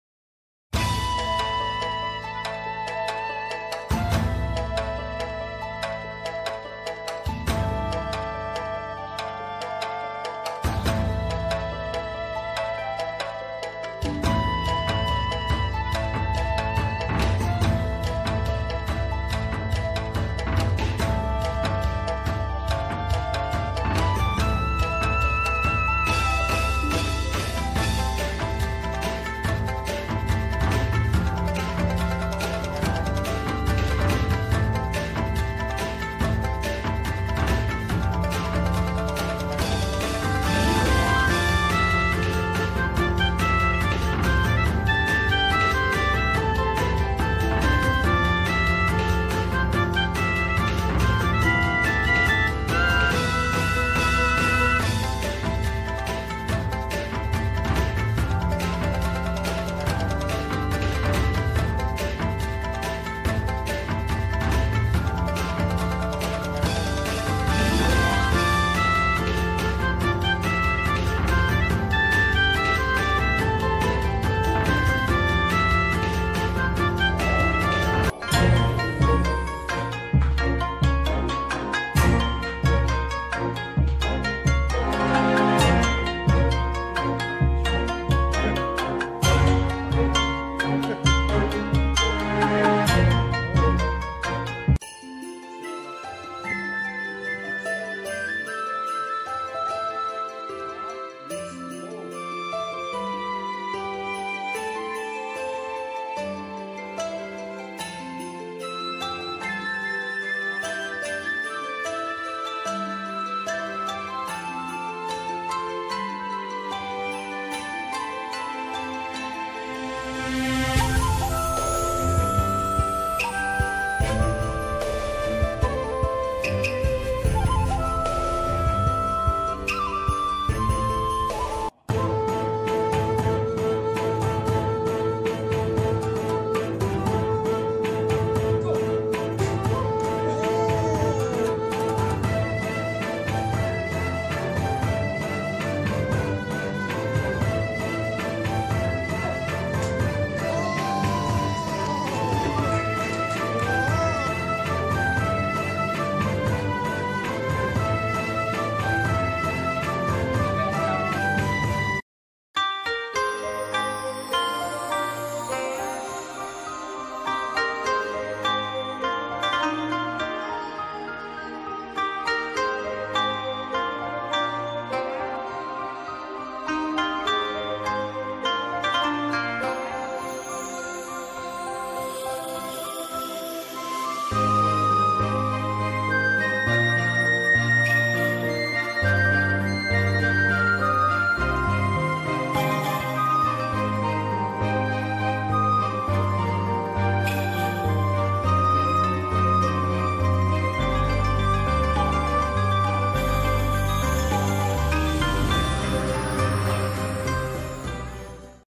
Iaido lesson with real Japanese sword to learn Samurai Spirit near the Imperial Palace in Tokyo!!
◆For detailed information of the Iaido lesson